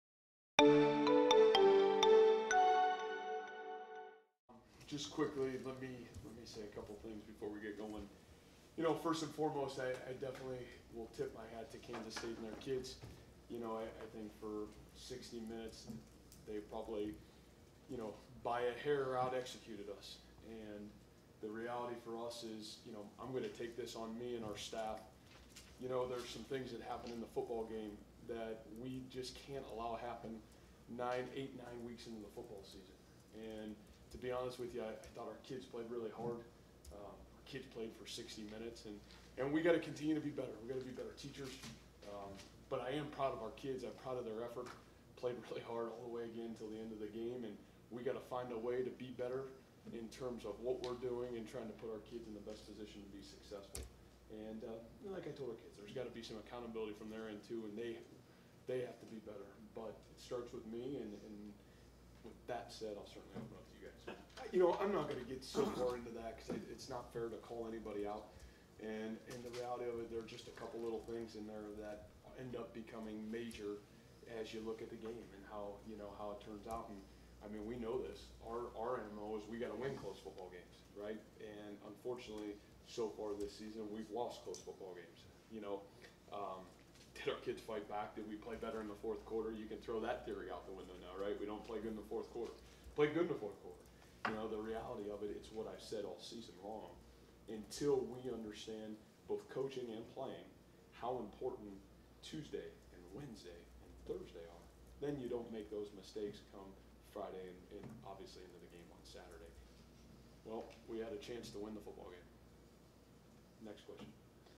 Matt Campbell after loss to Kansas State
Iowa State head coach Matt Campbell talks about his team's 31-26 loss to Kansas State on Saturday, Oct. 29, 2016. He goes over mistakes and Mike Warren's usage.